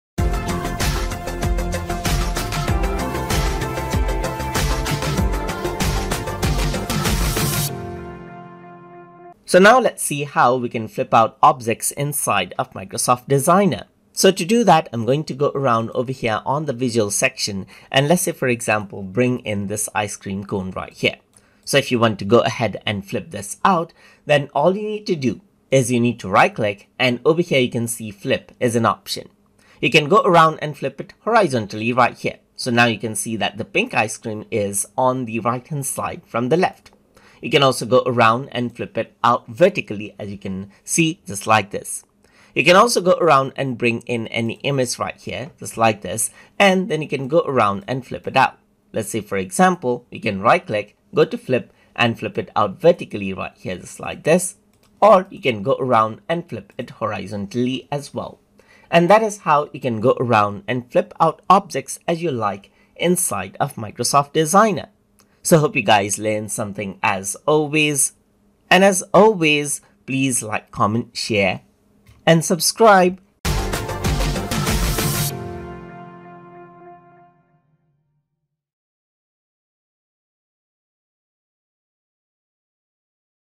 Microsoft Designer - Lesson 22 - Flipping Objects
In this tutorial, we will be discussing about Flipping Objects in Microsoft Designer

Learn the basics of using Microsoft Design including how to design, use generative AI, use templates and work with different layouts.

This lesson has been made using Microsoft Designer in Google Chrome Browser

If you like this video, here's our entire playlist of Microsoft Designer tutorials: